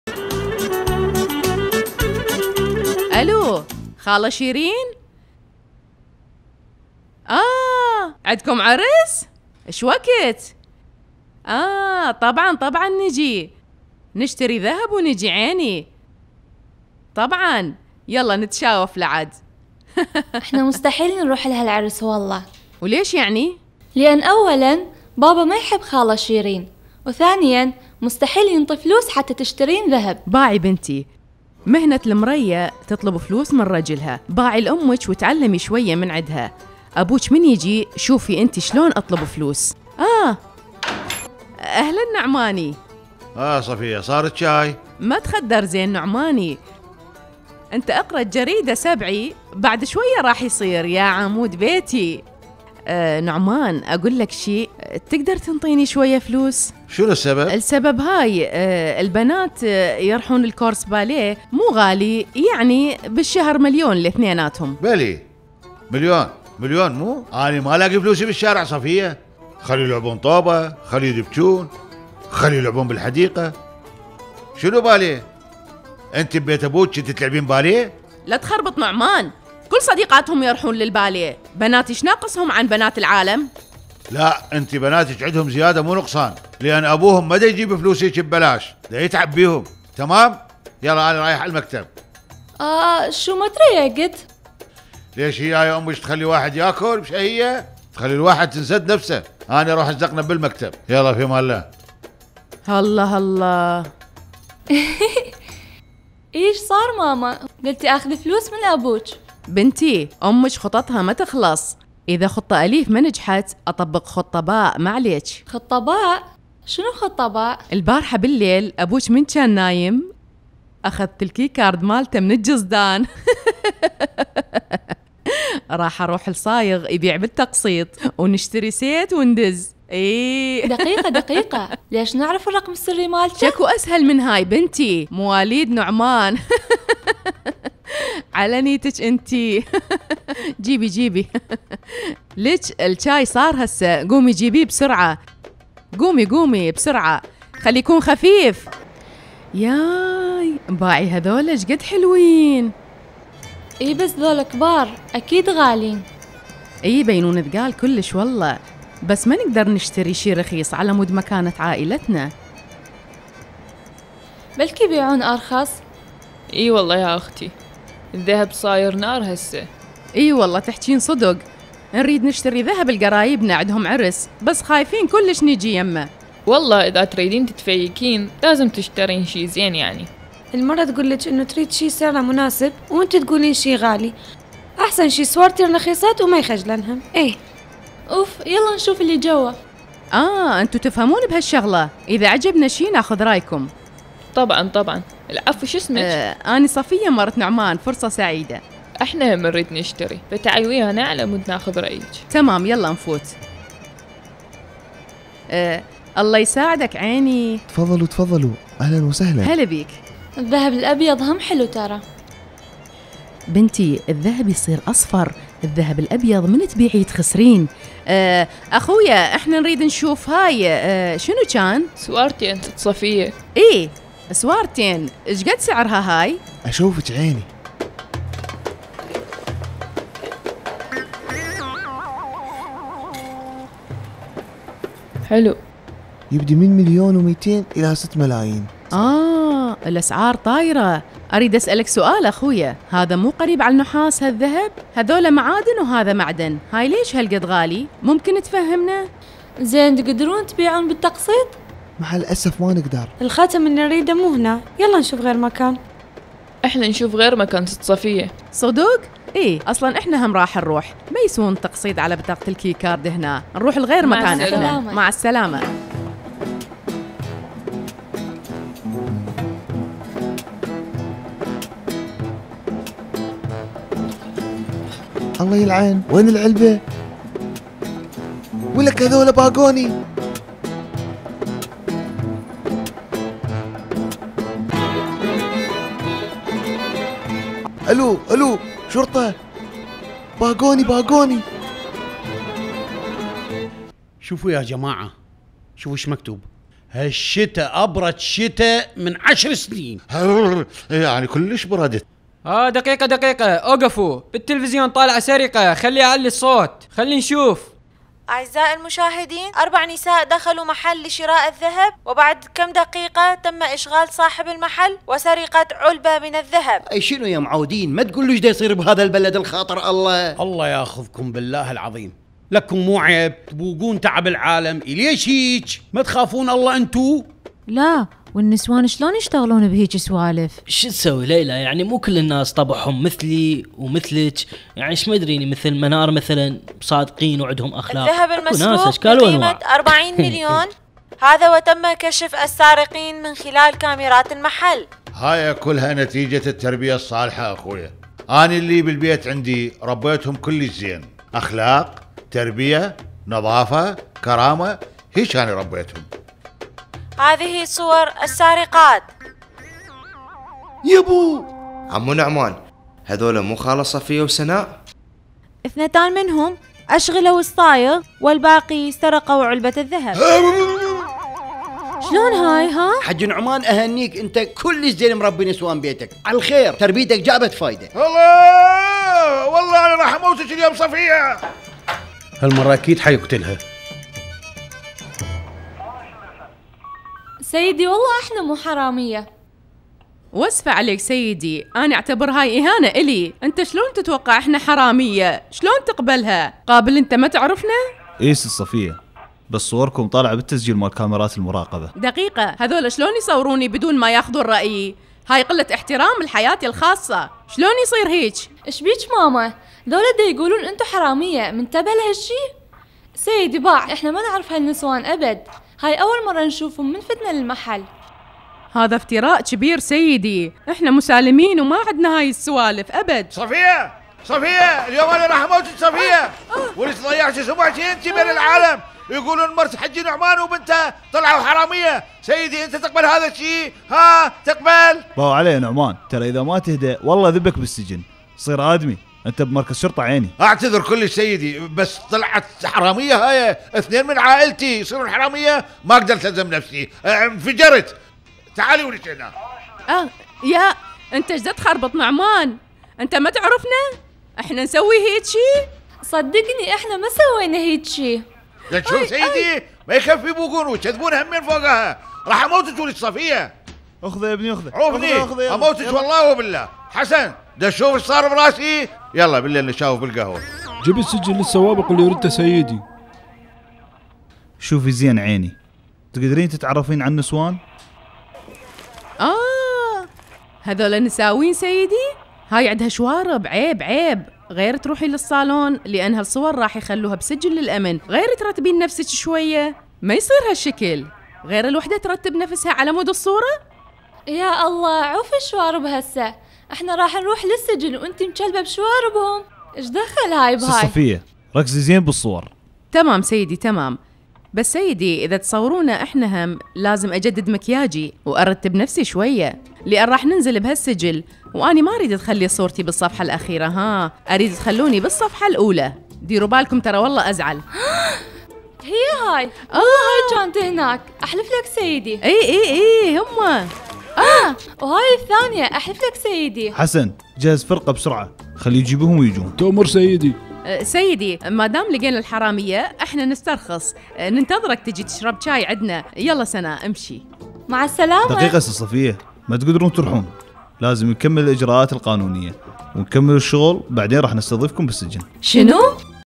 صفية تريد تشتري ذهب بالسر..شوفوا الحجي نعمان شنو سوه من عرف بالسالفة 😅
صفية تريد تشتري ذهب بالسر..
شوفوا الحجي نعمان شنو سوه من عرف بالسالفة 😅